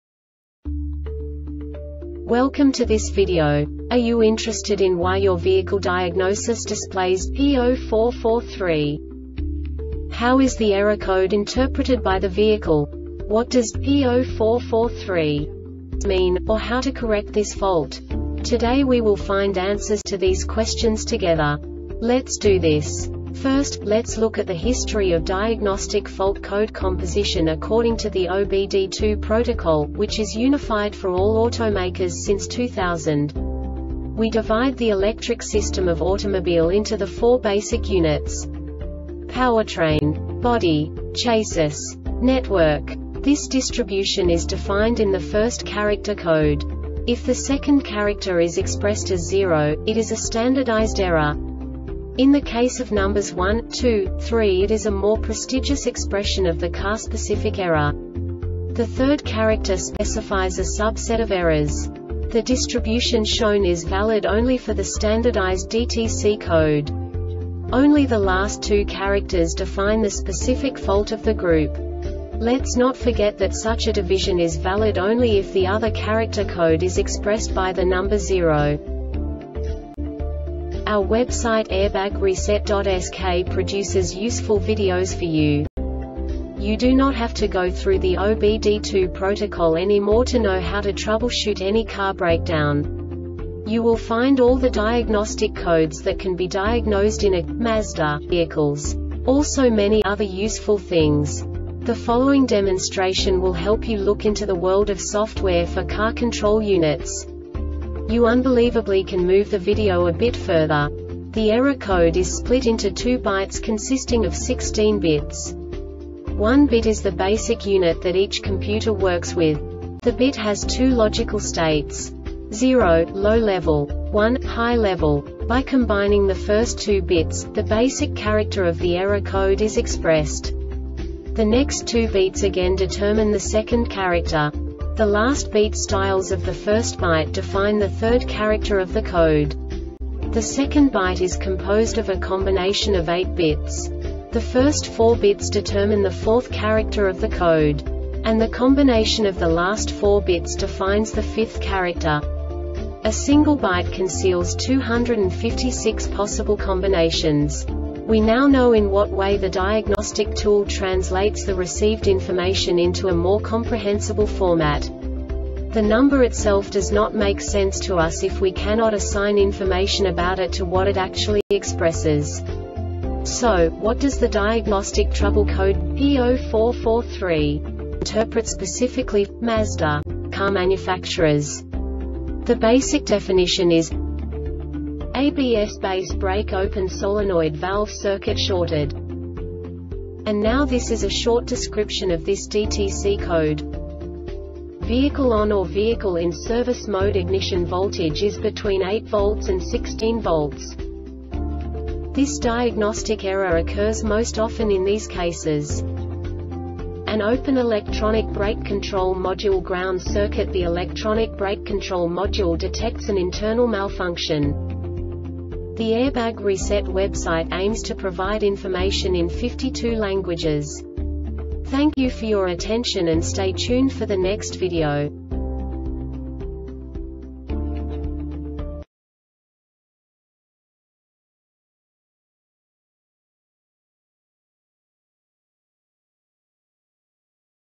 DTC Mazda P0443 Short Explanation
Contents:
0:21 Basic DTC analysis according to OBD2 protocol standard.
1:48 Insight into programming
2:58 A diagnostically understandable description of the Diagnostic Trouble Code
3:05 Basic error definition

Make:
Mazda
Code:
P0443
Definition:
ABS Base Brake Open Solenoid Valve Circuit Shorted
Description:
Vehicle ON or Vehicle in Service Mode.Ignition voltage is between 8 V and 16 V.
Cause:
An open electronic brake control module ground circuit.The electronic brake control module detects an internal malfunction.